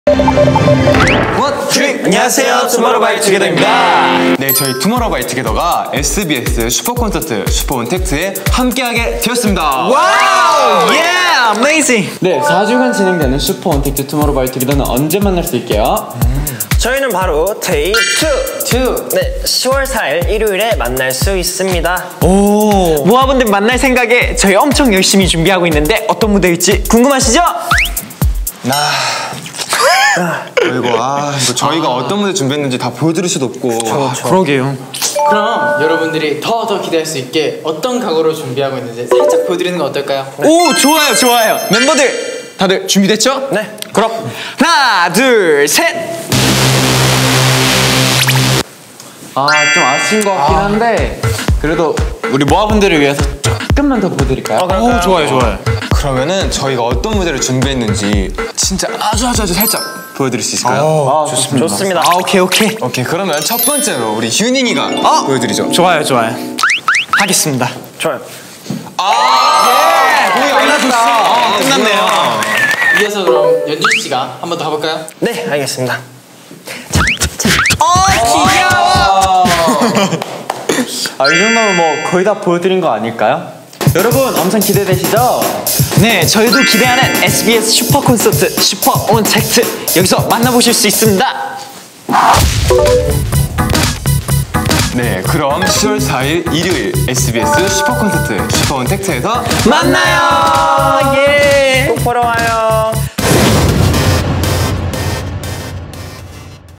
2020 SUPER CONCERT-SUPER ON:TACT!♥DAY2♥ TOMORROW X TOGETHER
2020 SUPER CONCERT-SUPER ON:TACT!♥DAY2♥ TOMORROW X TOGETHER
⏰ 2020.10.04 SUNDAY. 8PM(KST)
# TOMORROW X TOGETHER joined SUPER ON:TACT!
TOMORROW X TOGETHER who always try to make a better tomorrow,
SUPER ON:TACT will unfold another chapter of their dream!
Do you want to know what's coming?
Check it out on SUPER ON:TACT!
Sunday, Octover 4th, at SBS SUPER ON:TACT!